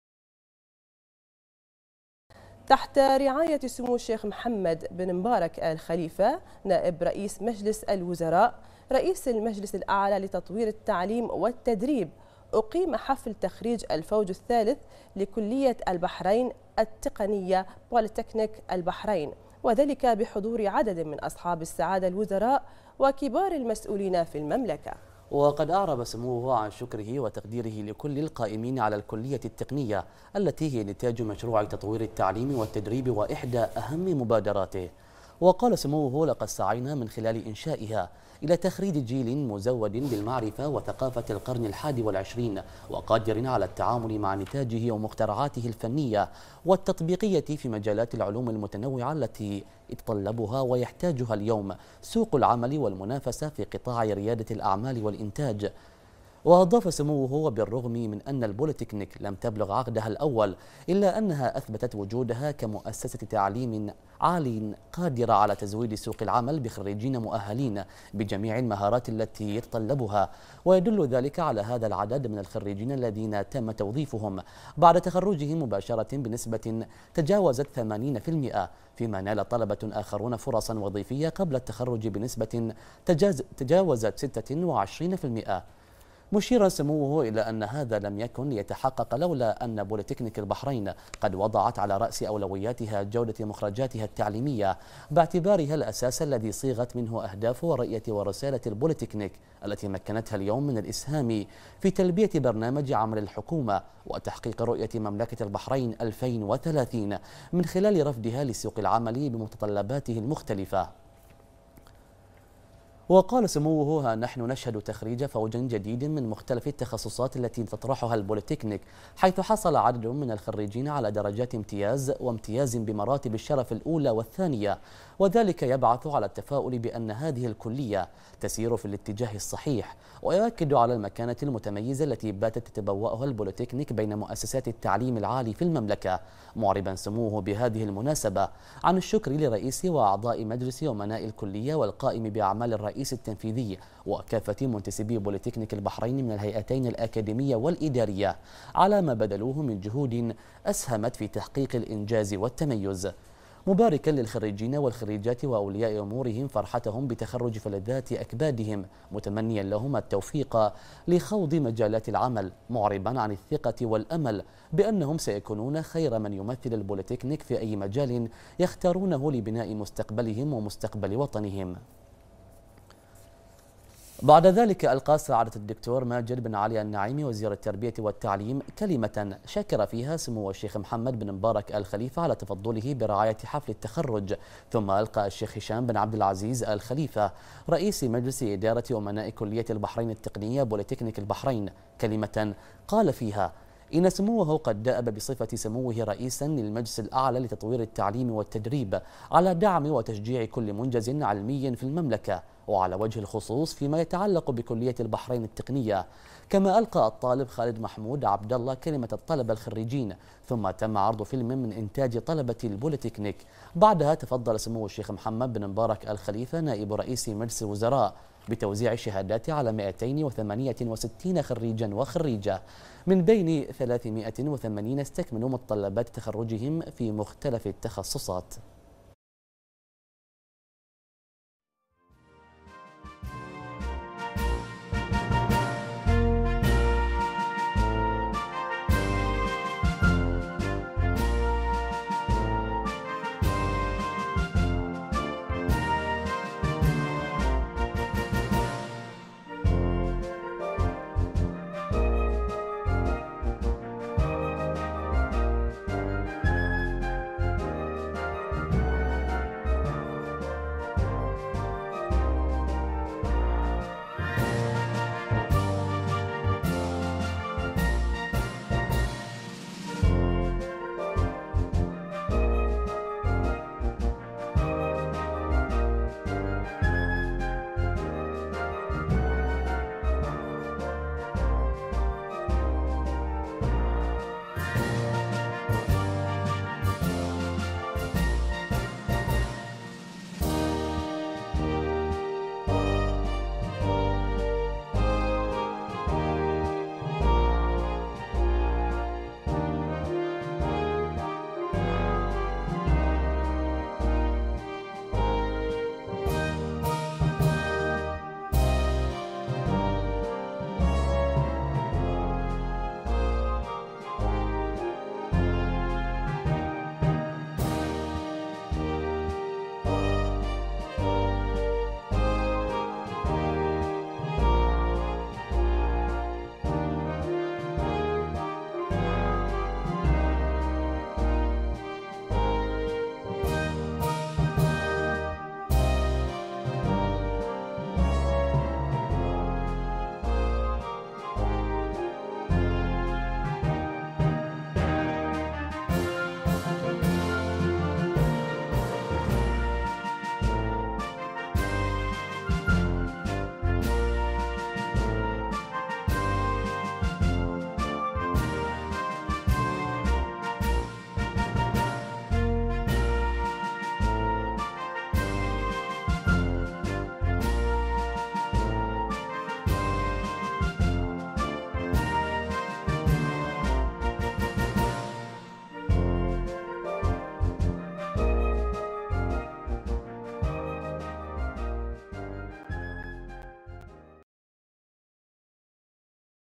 البحرين: سمو نائب رئيس مجلس الوزراء يرعى حفل تخريج كلية البحرين التقنية (بوليتكنك البحرين)
المنامة في 4 ديسمبر / بنا / رعى سمو الشيخ محمد بن مبارك آل خليفة نائب رئيس مجلس الوزراء، رئيس المجلس الأعلى لتطوير التعليم والتدريب حفل تخريج الفوج  الثالث  لكلية البحرين التقنية (بوليتكنك البحرين) الذي اقيم في قاعة الشيخ عبدالعزيز بن محمد آل خليفة بمقر جامعة البحرين في الصخير وذلك بحضور عدد من اصحاب السعادة الوزراء وكبار المسؤولين في المملكة.